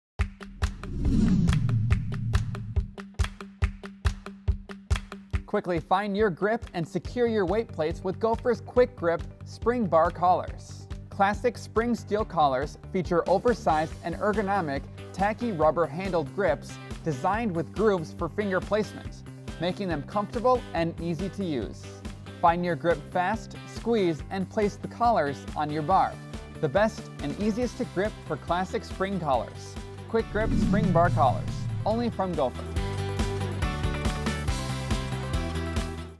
Steel Barbell Collars
These classic 2” spring steel bar collars have oversized ergonomic rubber-covered handles to make them more comfortable and easy to use. The handles themselves have a tacky texture, allowing users to easily put on and take off the barbell spring collars, even when their hands are slick and sweaty.